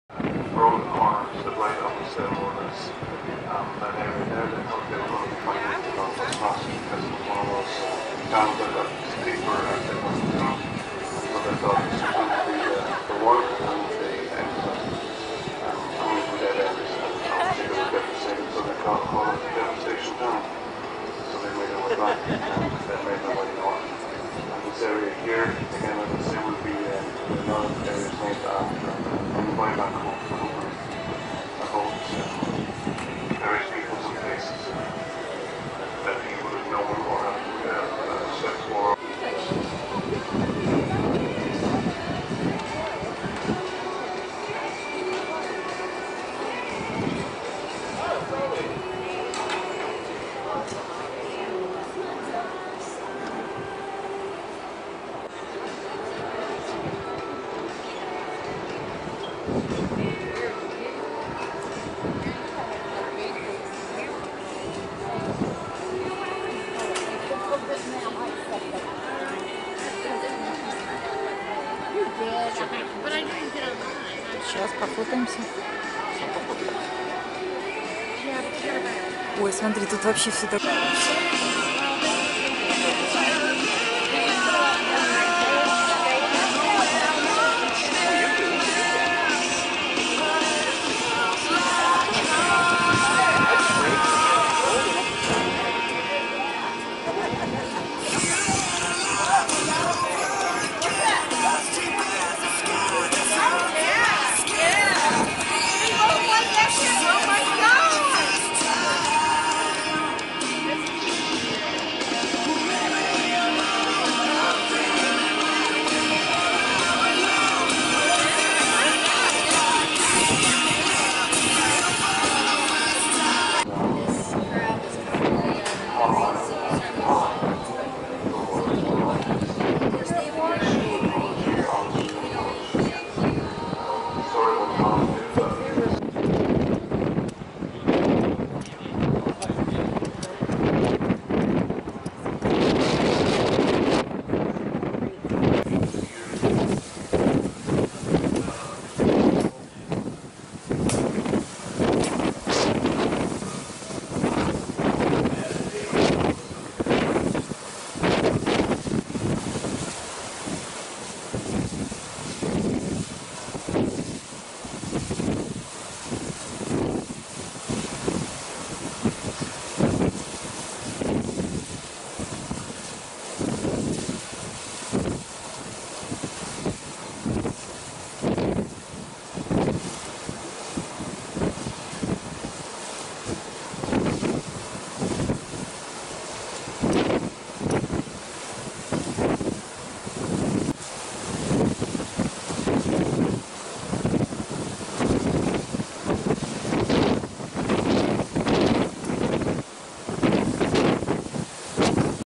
Trip round the world-2012. Cruise to Alaska. 2nd day. part 2
Кругосветка-2012. Круиз по Аляске. 2-й день. ч. 2
Целый день в море. Идем между островами и материком. На 2:28 - самый узкий проход. My video. 8'2012.